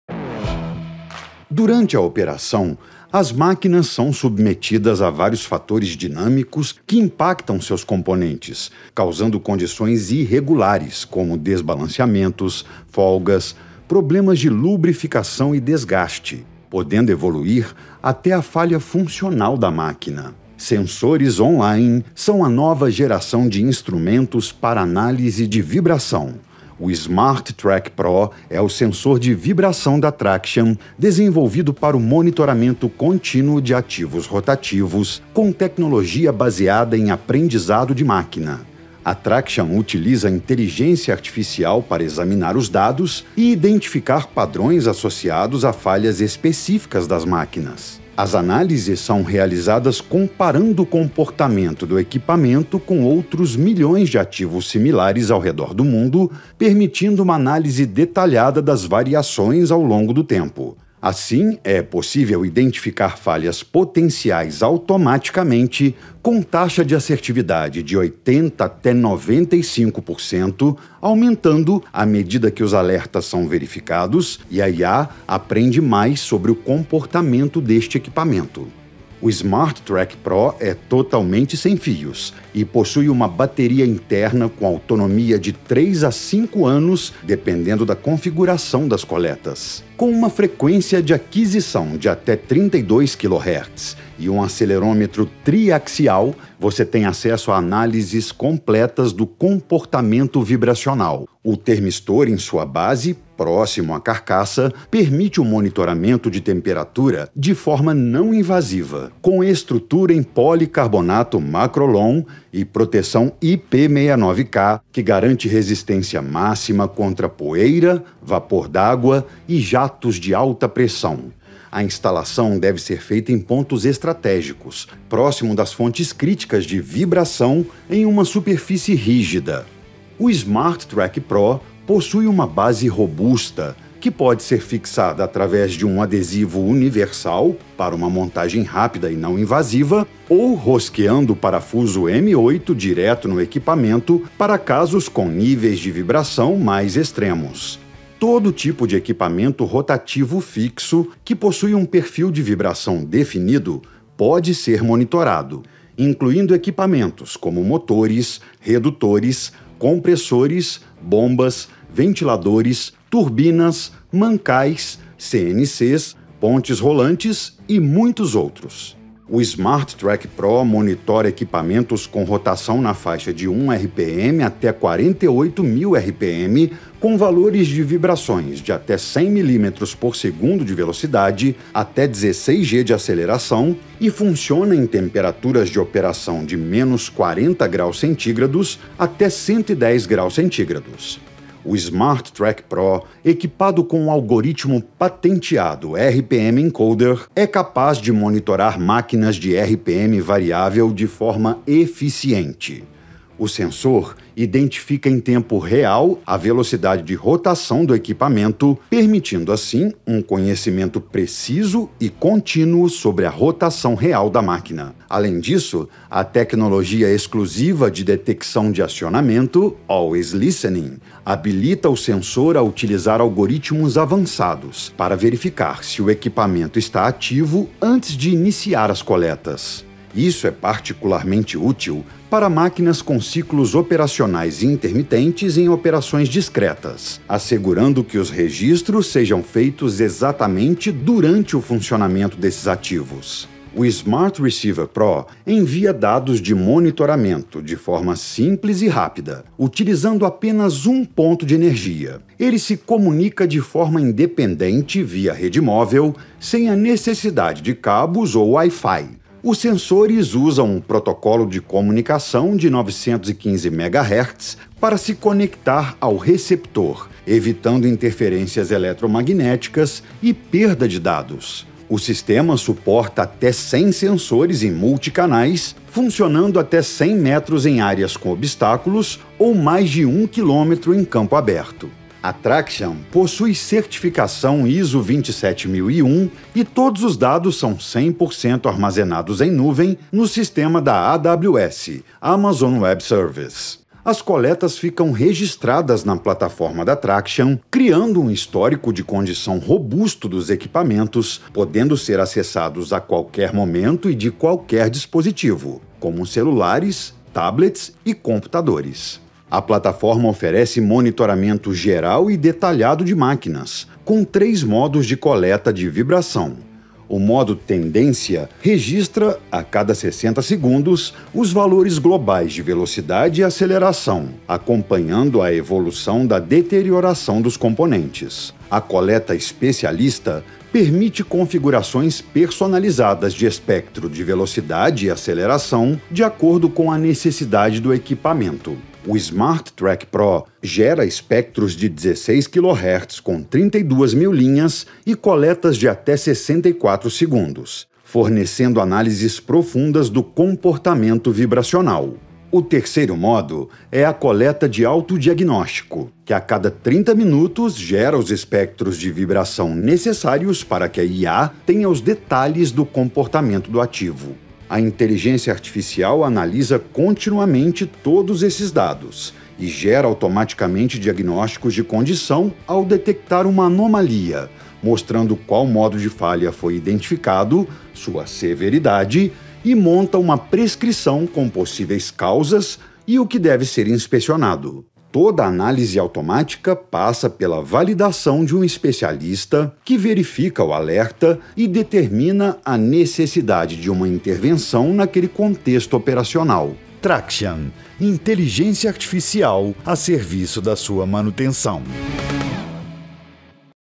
Revolução no Monitoramento Online com IA: Sensor Smart Trac PRO
Descubra como o monitoramento online com IA do sensor Smart Trac PRO está revolucionando a manutenção preditiva. Este sensor de última geração utiliza Inteligência Artificial avançada para elevar a precisão do monitoramento de vibração a um novo patamar.

Com o Smart Trac PRO, sua operação terá:

• Autodiagnóstico em +70 tipos de falhas através de IA;
• Manutenção Assistida por IA para uma atuação certeira;
• Novo protocolo de comunicação proprietário para coleta de dados;
• Tecnologia única Always Listening™ para detecção de acionamento;
• Mobilidade para Especialistas realizarem análises em campo;
• Monitoramento preciso de ativos de baixa rotação.

O monitoramento online com IA do Smart Trac PRO irá maximizar a disponibilidade dos seus ativos através de diagnósticos automatizados e uma manutenção preditiva de alto nível.

Esteja à frente do mercado com nosso novo sensor e garanta a máxima performance da sua operação com o sensor que está definindo o futuro do monitoramento de ativos industriais. Veja de perto como o monitoramento online com IA irá transformar a sua manutenção.